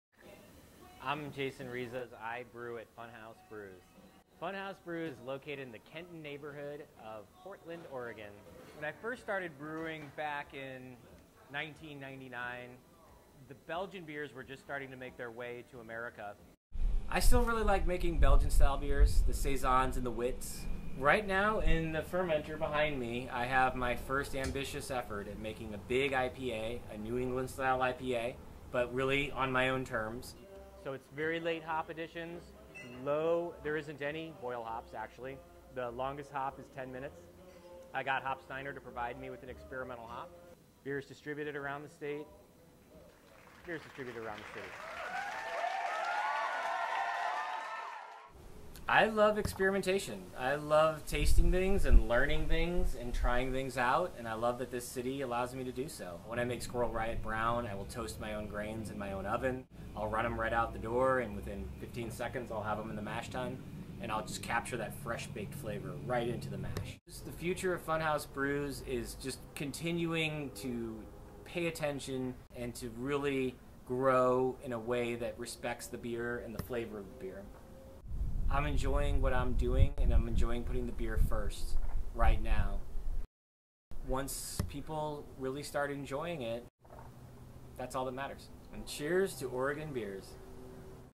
Jason Rizos Funhouse Brews Project
We made this video for Jason Rizos of Funhouse Brews.  It was tough to cut it down to 90 seconds with so much good material.